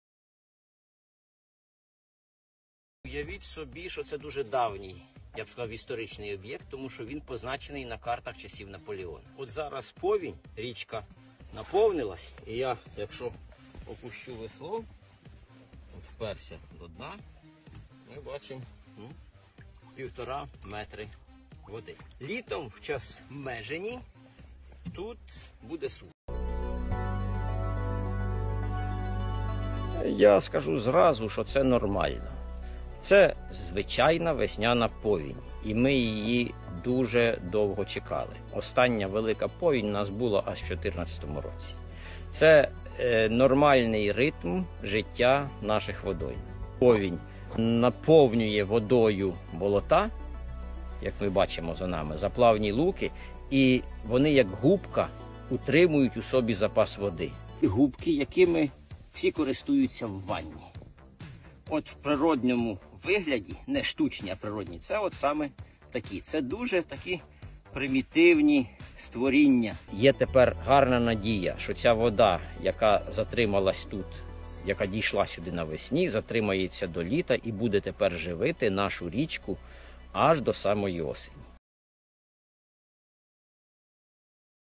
Повінь на Удаї.
"Річки міліють, пересихають",- таке часно можна чути останнім часмо у різних куточках України. Ми пережили десятиріччя мінімуму водності річок. Багато в чому тут є вина надмірного розорювання земель. Зараз з'явилися ознаки відновлення гідрологічного режиму. Болота, які наповнюються весняними водами, будуть поїти наші річки протягом всього літа, вбережуть від посухи, сприятимуть високим врожаям. Відео створене за матеріалами Полтавського обласного телеачення